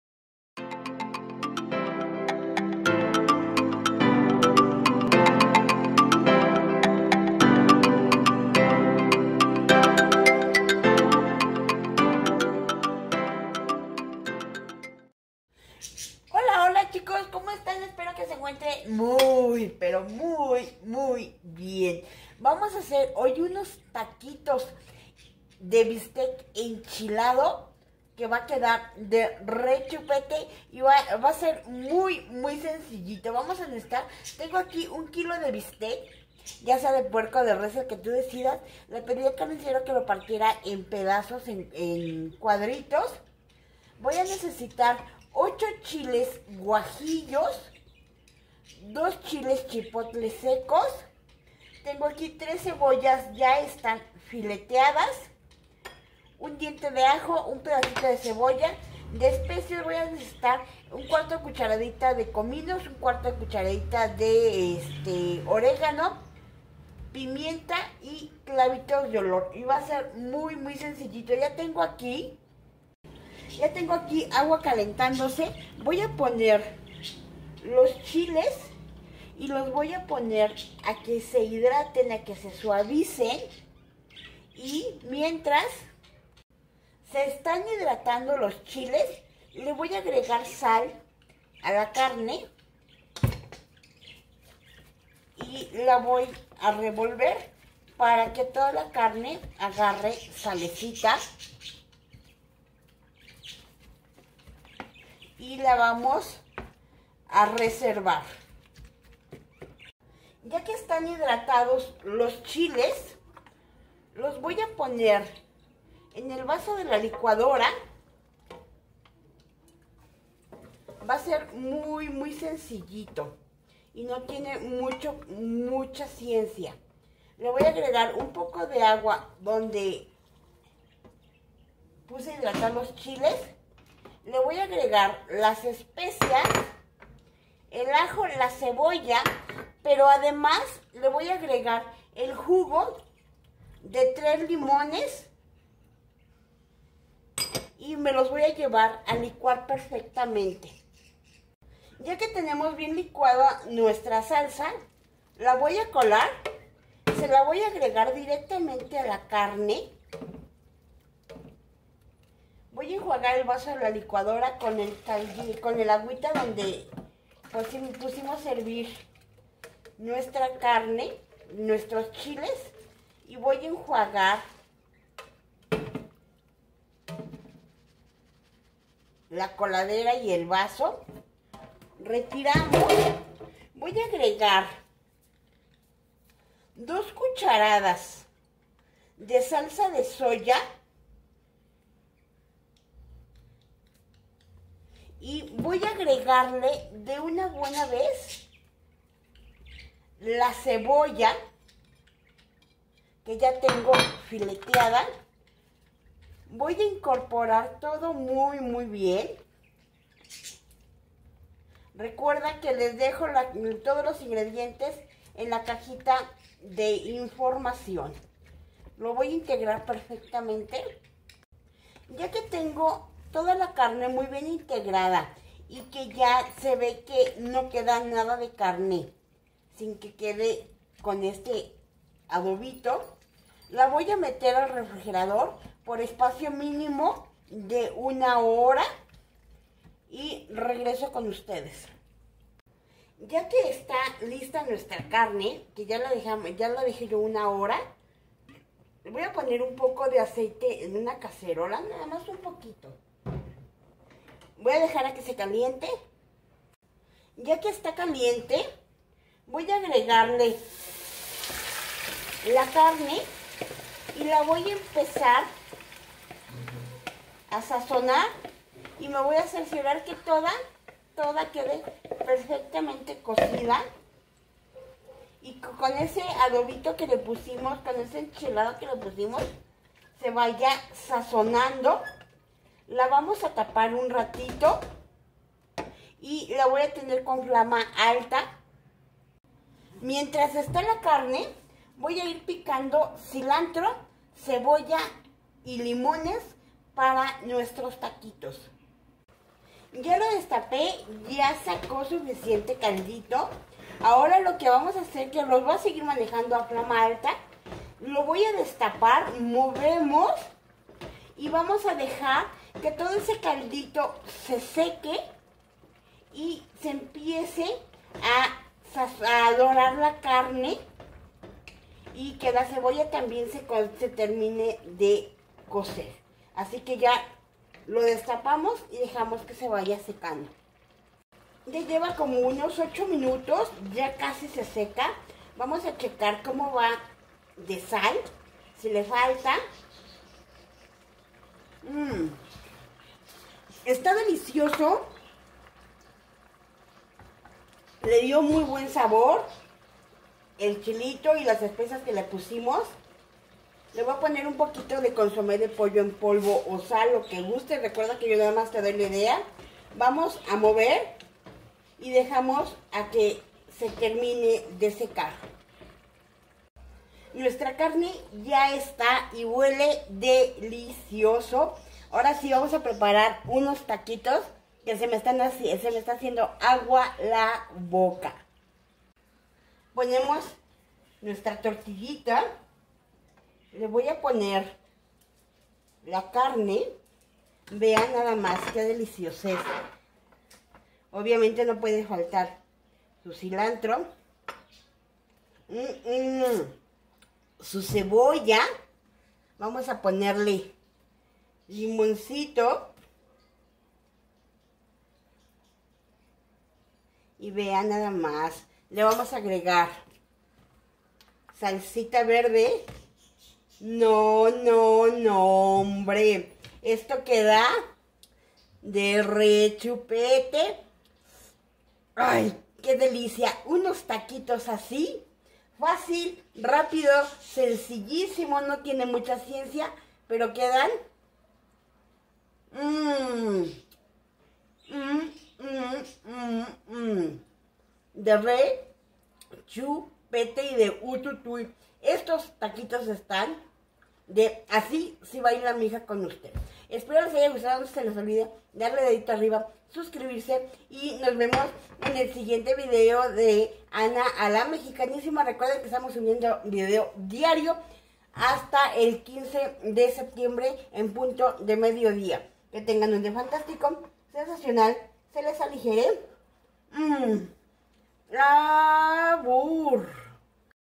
TACOS DE BISTEC ENCHILADO
INGREDIENTES
1 K bistec picado
3 cebollas fileteadas
8 chiles guajillos
2 chiles chipotles
1 diente ajo
1/4  cebolla
pimienta, clavos,orégano, cominos
2 c salsa soya
3 limones jugó